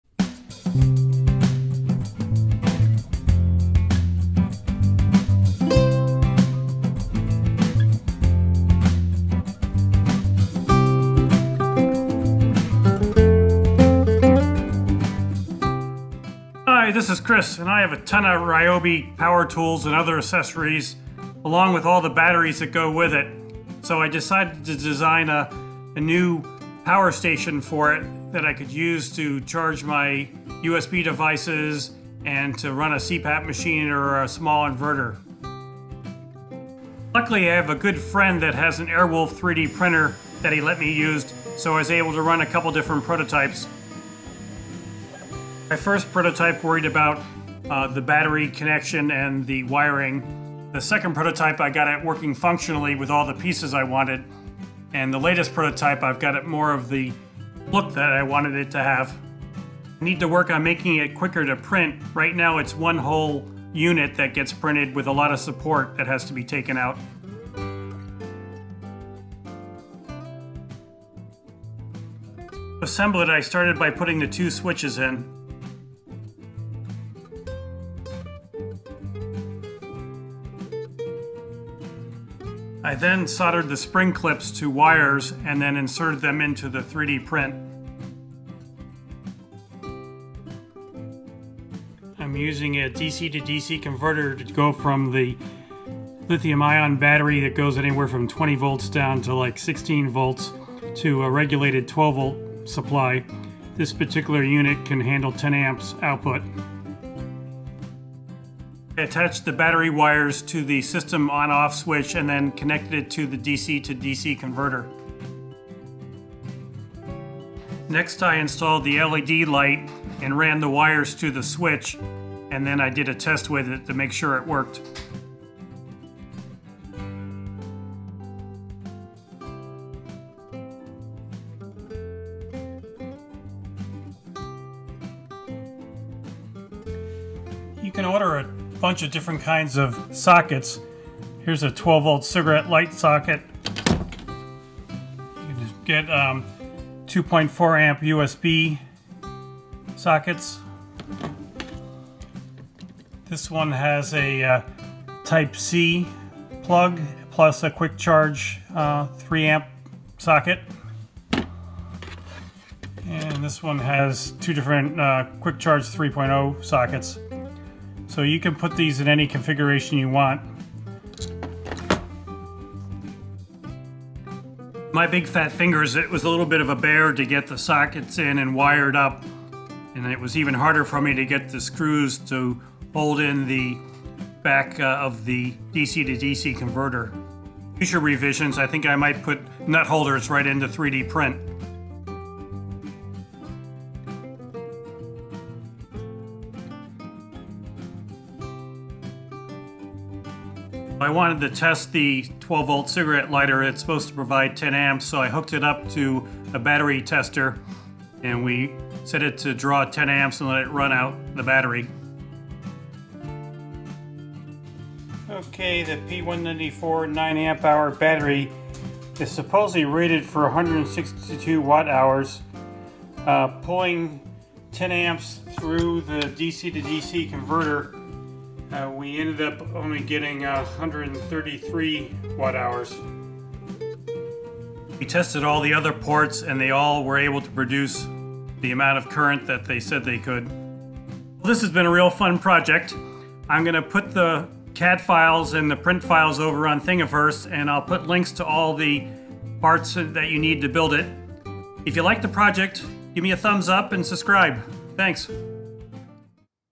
DIY Ryobi 18V One+ Powerstation V2
We have a ton of Ryobi tools and batteries. It dawned on us that we could build a powerstation using the replaceable Ryobi 18V batteries.

Here is our second Prototype. It has two 2.4A USB ports, 1 QC 3.0, 1 Type C port, 1 12V cigarette socket, 1 LED light

We put the case CAD model and STL model on Thingiverse

Here are the parts we used: (Affliliate links):

Carefully check the Amazon products. Lately I've found if Amazon is out of stock, they change the link to point to another similar/but wrong product!!!

* Ryobi 18V 9Ah Lithium battery
* Bestek 300Watt Pure Sine wave AC inverter
* Ryobi 150-Watt Powered Inverter, with 2 USB Ports and One 120-Volt Outlet
* Ryobi 18V One+ Dual Chemistry Vehicle charger
* DC-DC 10A 12V buck converter
* cigarette lighter socket
* dual 2.4A USB ports
* QC 3.0 and type C socket
* toggle switch
* LED puck light
* Battery Leaf springs
* 2.85mm ABS Filament 1kg for 3D Printers, Fluorescent Green
* Flexible 12 AWG Stranded Copper Wire
* 3/4" strap / Heavyweight Polypropylene Webbing